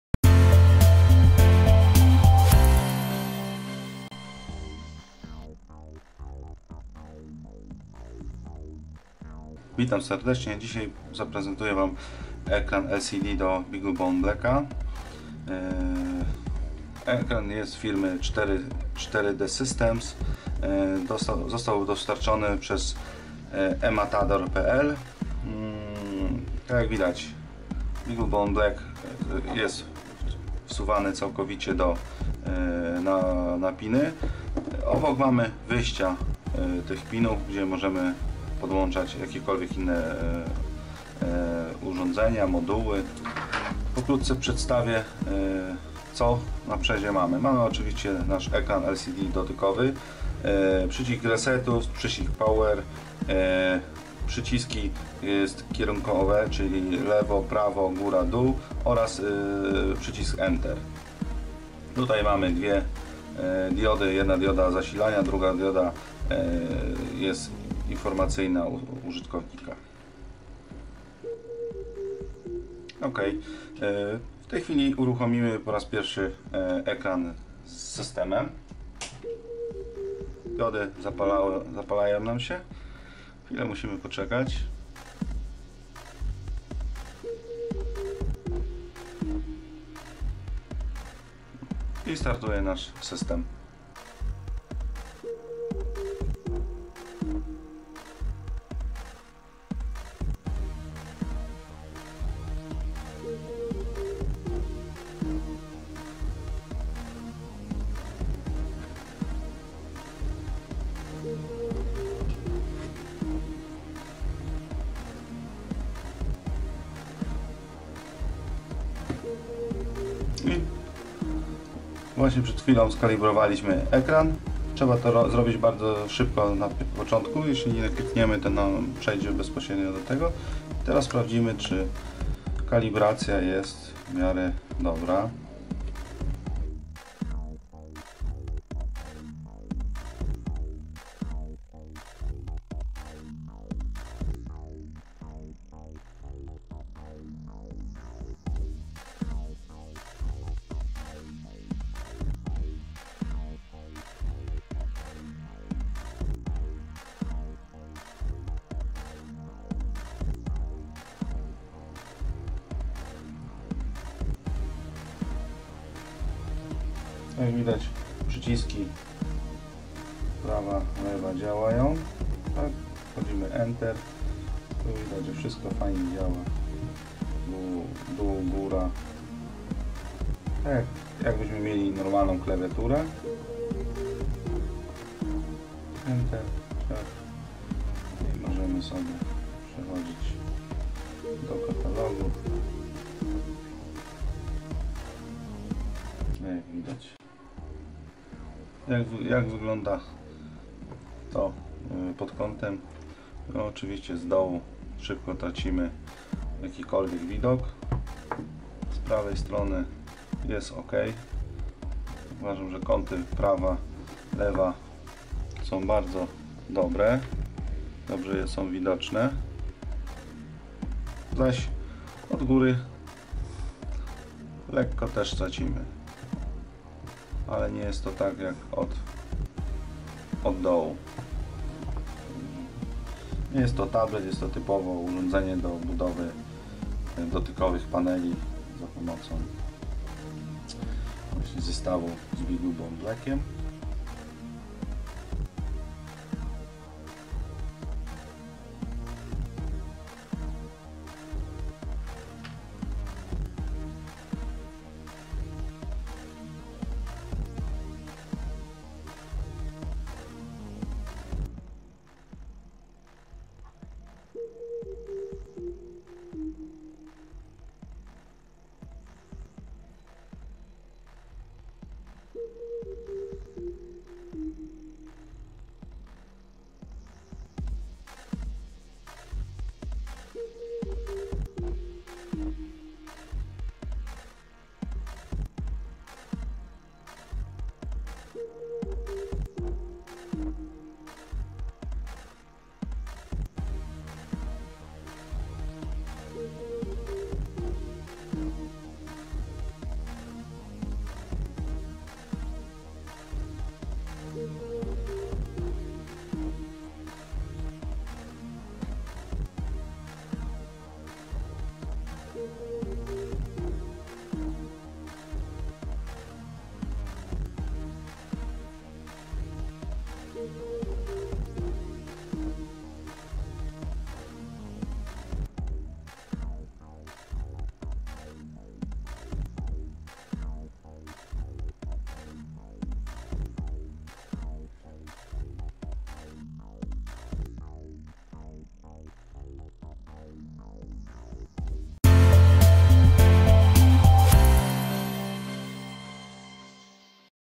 Moduł 4DCAPE-70T dla BeagleBone Black
Moduł 4DCAPE-70T to 7 calowy dotykowy wyświetlacza LCD TFT dla BeagleBone Black, który otrzymaliśmy od sklepu internetowego ematador.pl. Moduł działa od od razu po podłączeniu do płytki poprzez złącza GPIO oraz posiada przyciski nawigacyjne. Dzięki wyprowadzeniom wideo na złączach GPIO w Beaglebone Black, nie potrzebujemy dodatkowych kabli do podłączenia urządzeń ze sobą, a sam dostęp do pinów jest dostępny z tyłu ekranu.